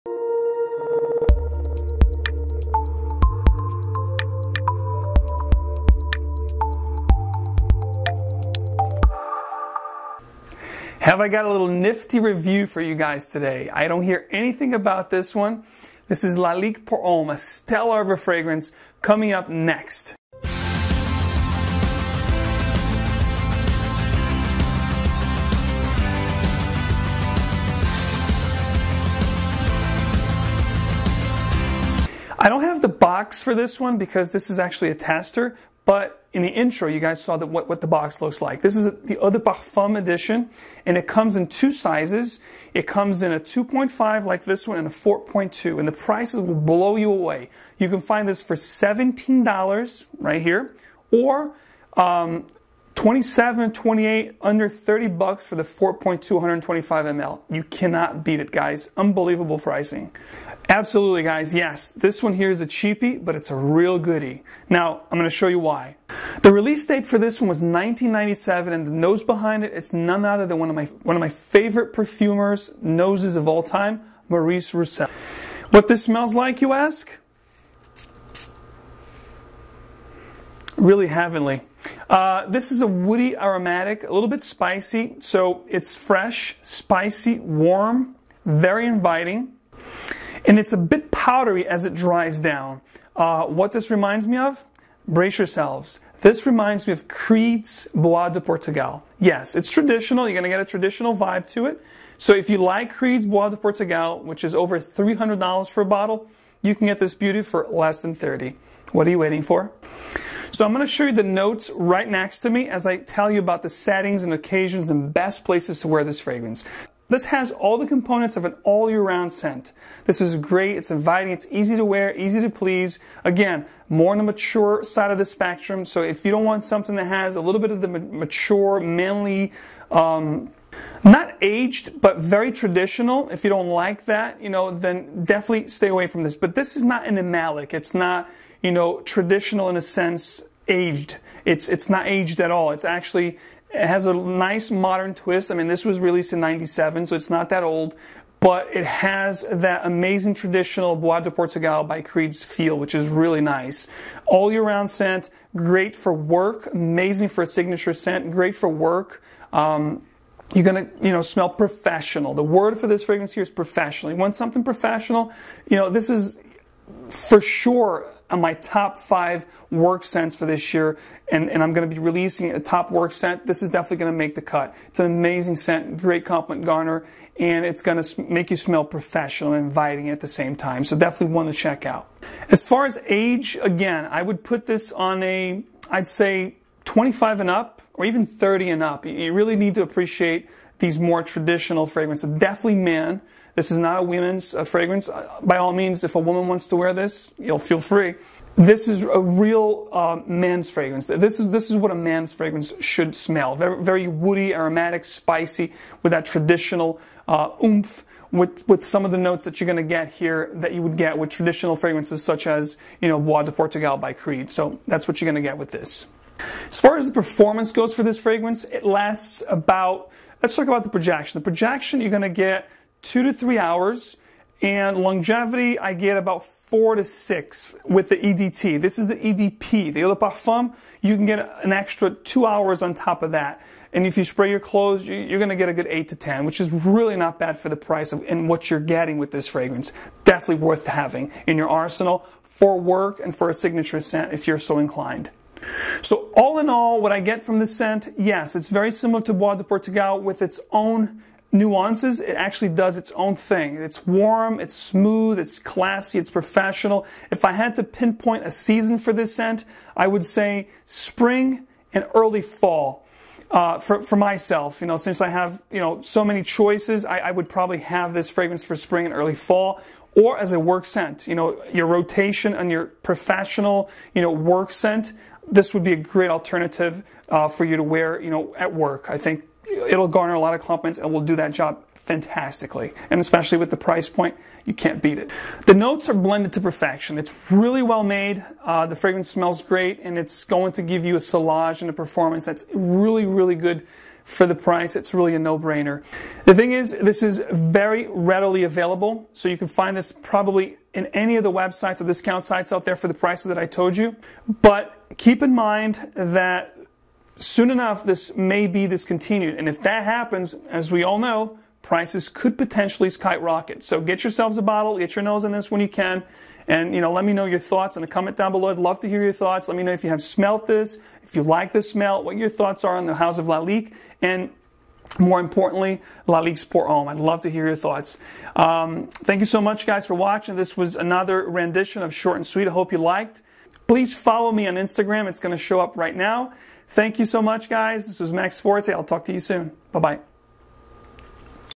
Lalique Pour Homme (1997) | Short & Sweet Review | Hidden Gems Series
Lalique Pour Homme | Short & Sweet Review
About this fragrance:
A softer and easier to wear alternative to Creed's Bois du Portugal. Traditional scent with a modern twist. Released in 1997 this one is awesome for everyday wear (signature scent) does wonders in the office/work setting. Easy to please and a compliment garner. Lalique Pour Homme Eau de Parfum is definitely worth checking out, especially due to its amazing price tag. All-in-all a word to describe this scent: PROFESSIONAL!
Final Rate: ✭✭✭